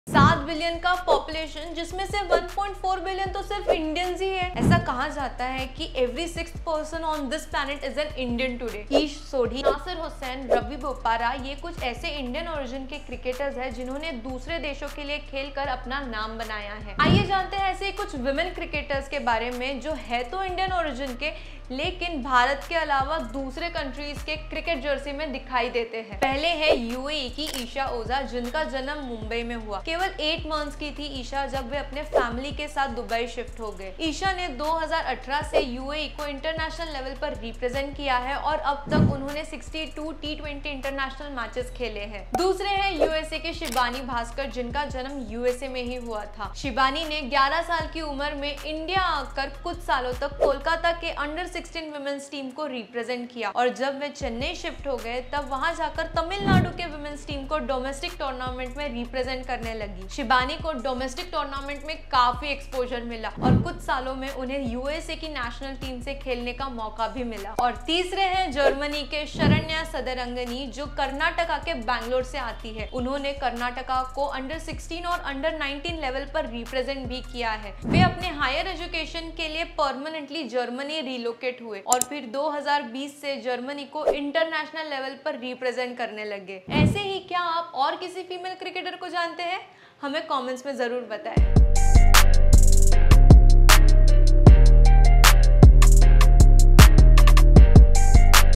Born in India, played for some other Country 😮 | Female Cricket
Ish Sodhi, Nasser Hussain, Ravi Bopara are some of the Indian-origin cricketers have made a name for themselves by playing for other countries. Watch the video to find out about some women cricketers of Indian origin who represent countries other than India in cricket jerseys.